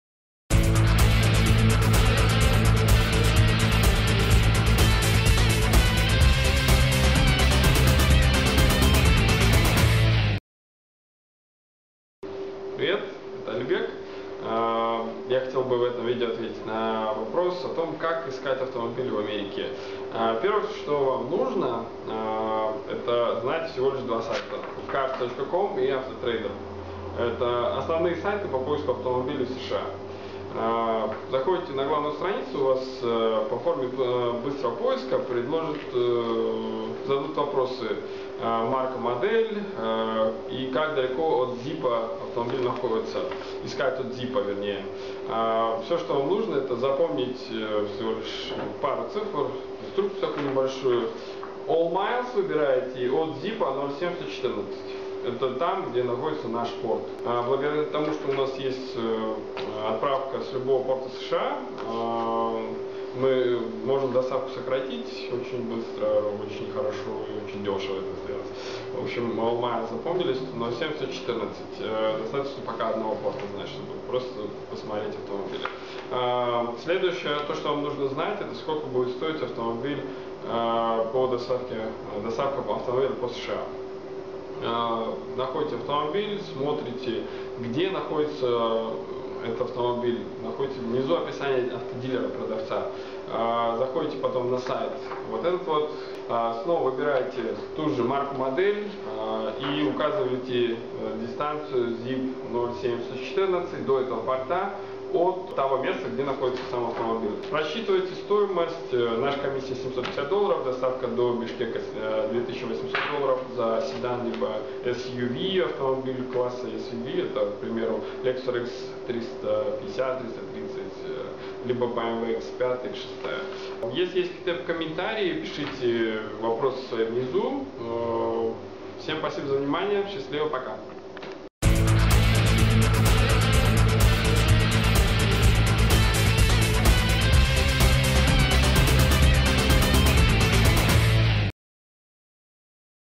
Инструкция как найти и как купить автомобиль в Америке (США)
Привет! Это Алибек. Я занимаюсь поиском и доставкой авто из США на заказ. В этом видео я подготовил инструкцию как найти автомобиль на сайтах Америки. Буду рад знать если это видео вам чем-то смогло помочь и вы смогли для себя найти правильный автомобиль из Америки.

Стоимость нашей комиссии $750
Доставка одного автомобиля до г. Бишкек $2800